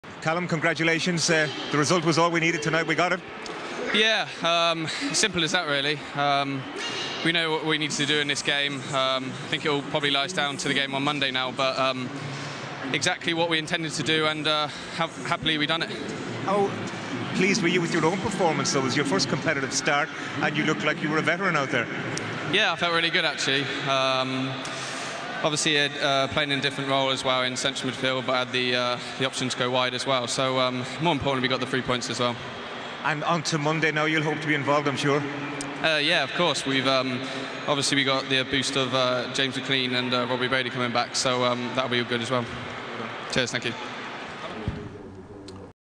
Republic of Ireland v Moldova - post-match interview - Callum O'Dowda (6/10/17)
Republic of Ireland winger Callum O'Dowda looks ahead to the final group match against Wales following a 2-0 win over Moldova at the Aviva Stadium in Dublin.